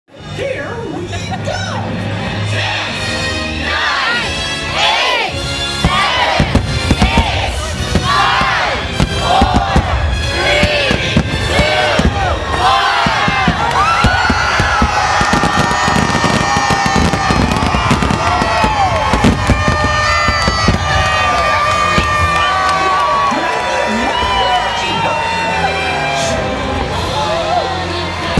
Walt Disney World Magic Kingdom 2017 New Year's Eve Celebration - Countdown to 2017
This is the famous New Year's Eve fireworks countdown to the new year celebration at Magic Kingdom!